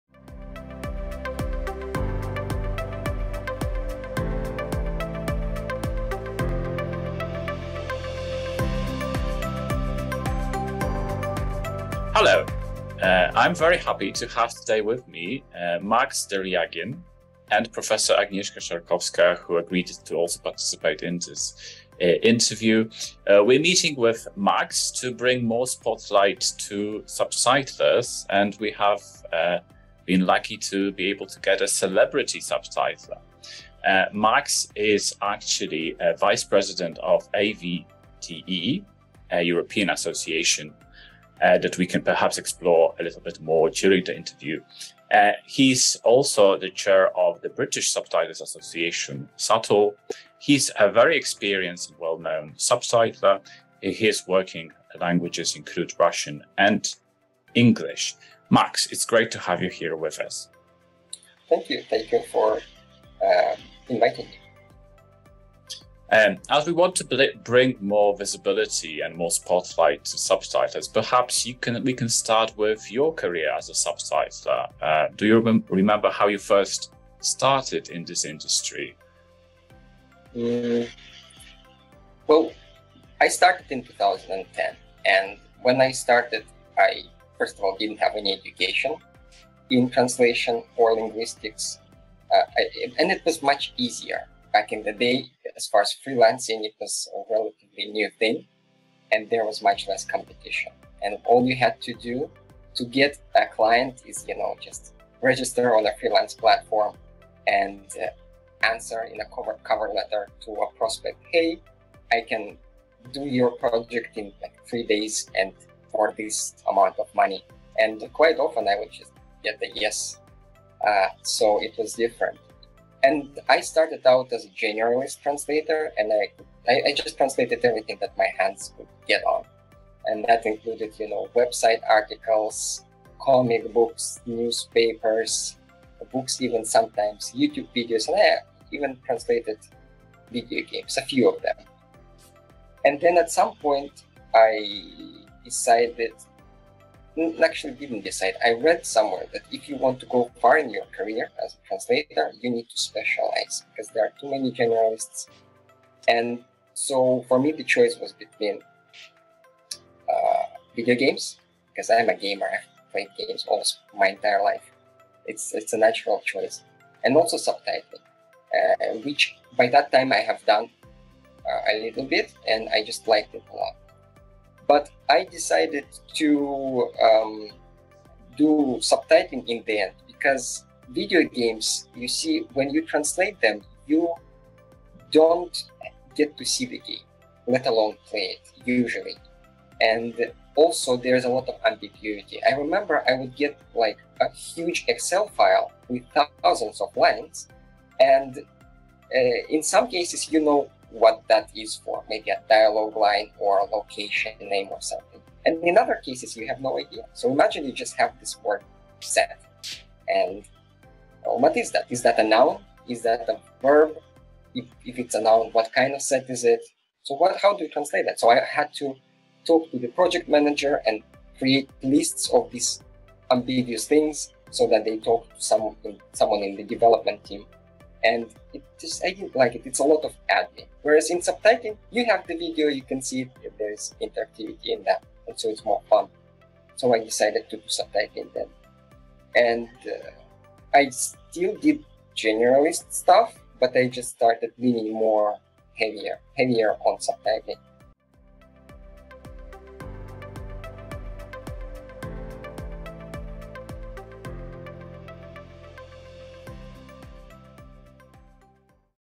PART 1: How did you get started as a subtitler? - Interview with Max Deryagin
It's time for the subtitlers to shine in the spotlight! We start this series by interviewing Max Deryagin, an experienced subtitler, who is very well-known in Europe. (You will actually hear Łukasz call him a "celebrity subtitler".) Max is the chair of the British Subtitlers’ Association SUBTLE, and vice president at AudioVisual Translators Europe (AVTE). His working languages include Russian and English.

In PART 1, Max tells Łukasz Dutka and Agnieszka Szarkowska how he started his career in subtitling. Was it easy to get into media localization at that time?

Want to learn more about Max Deryagin?